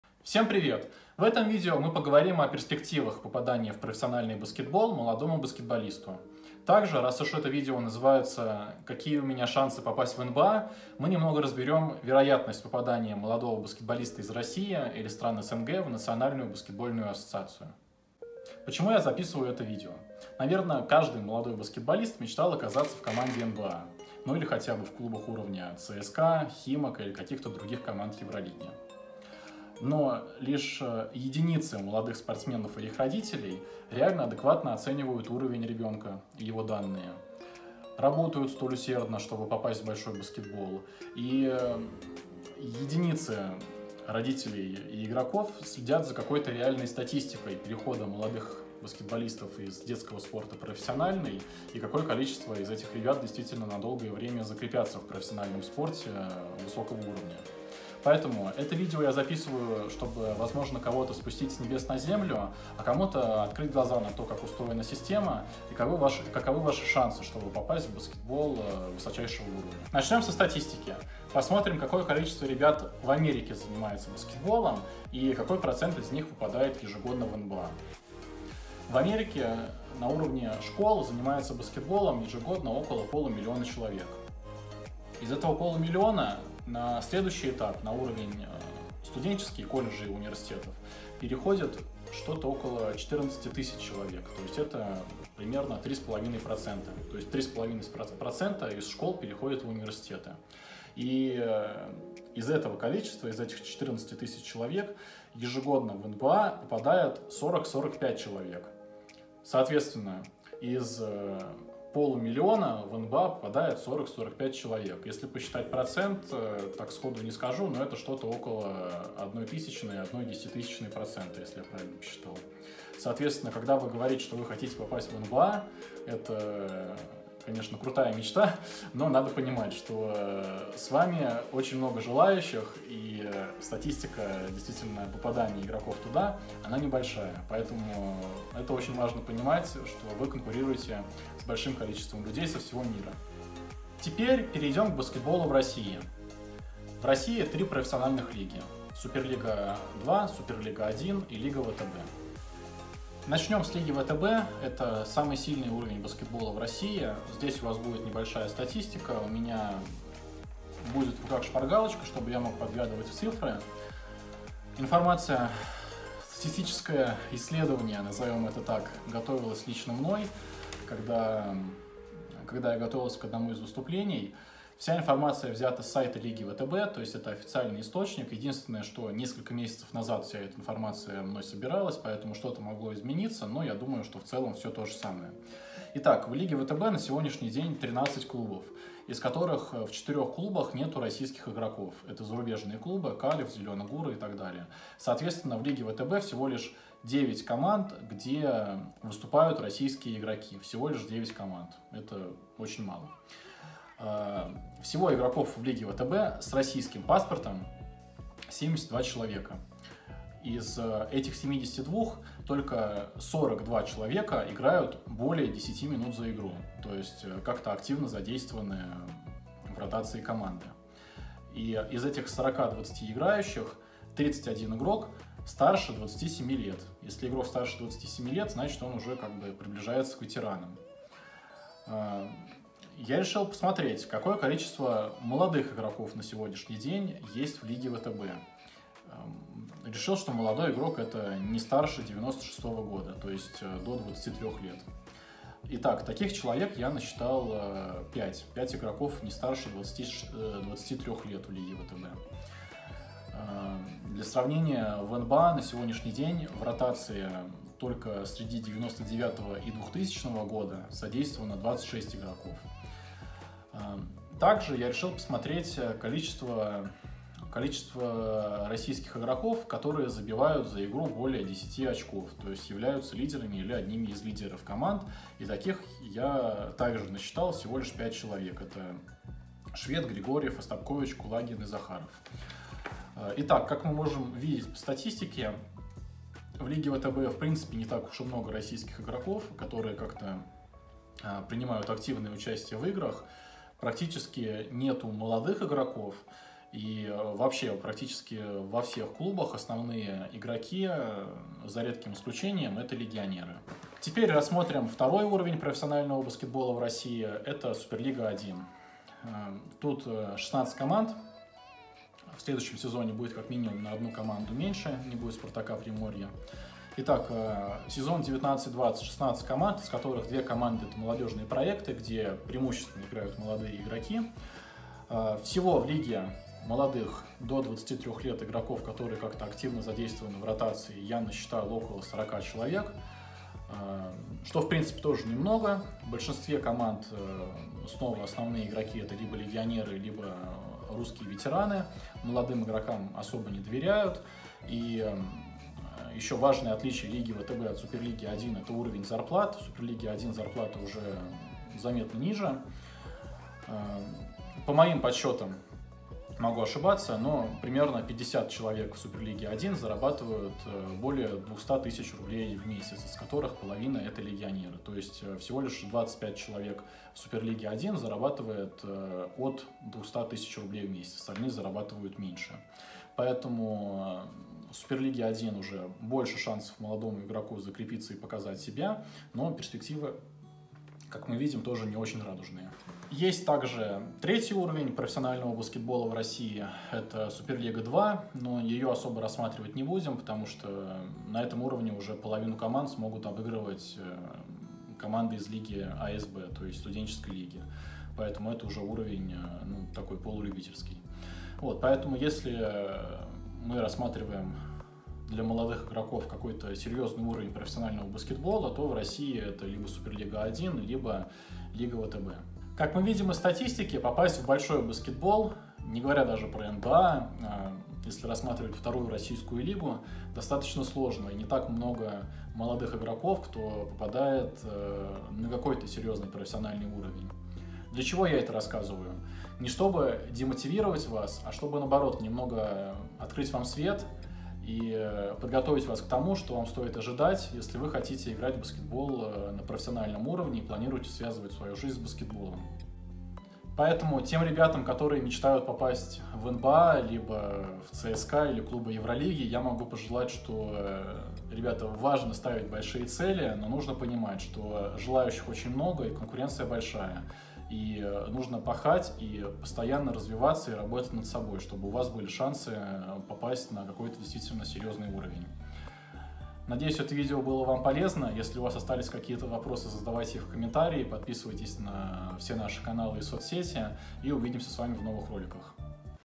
Как попасть в НБА?
Все молодые баскетболисты мечтают попасть в НБА или хотя бы в команды уровня ЦСКА или других клубов Евролиги.

В этом видео мы разберем:
- Какие у вас шансы попасть в НБА?
- Сколько молодых игроков задействовано в Лиге ВТБ
- Какие у вас возможности заиграть в российских профессиональных лигах

- спортивное агентство Pro Futuro Sports. Спорт и обучение в США и Европе.